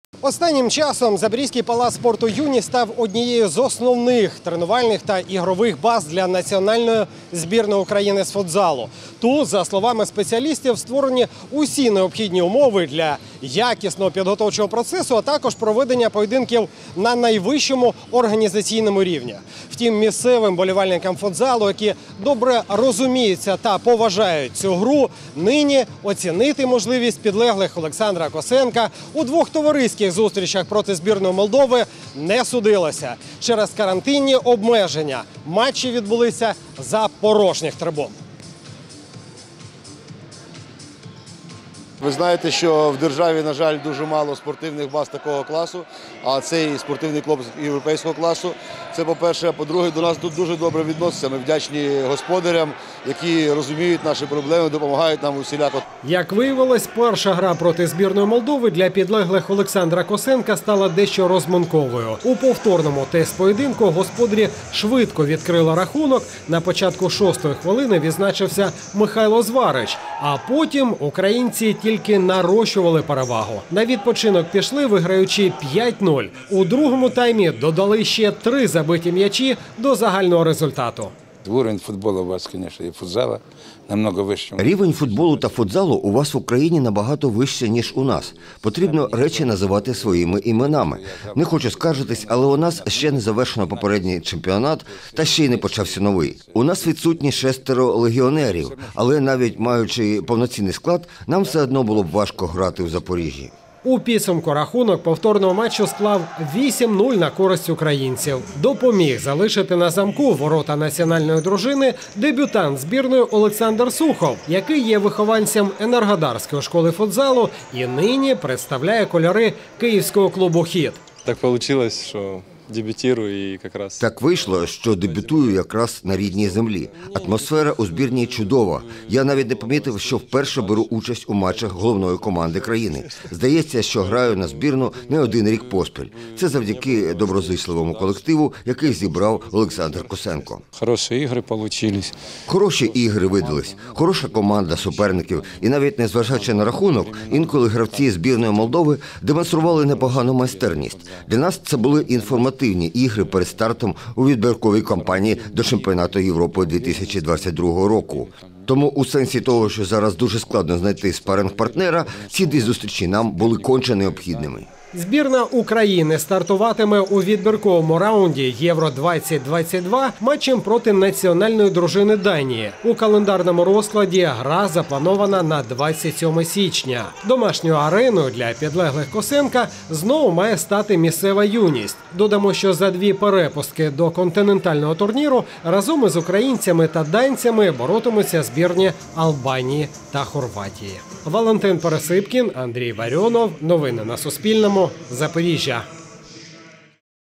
Збірна України з футзалу | Новини | 10.12.2020
Футзалісти збірної України у Запоріжжі провели два контрольних матчі проти національної дружини Молдови. Зустрічі відбулись восьмого та дев’ятого грудня у місцевому Палаці спорту «Юність».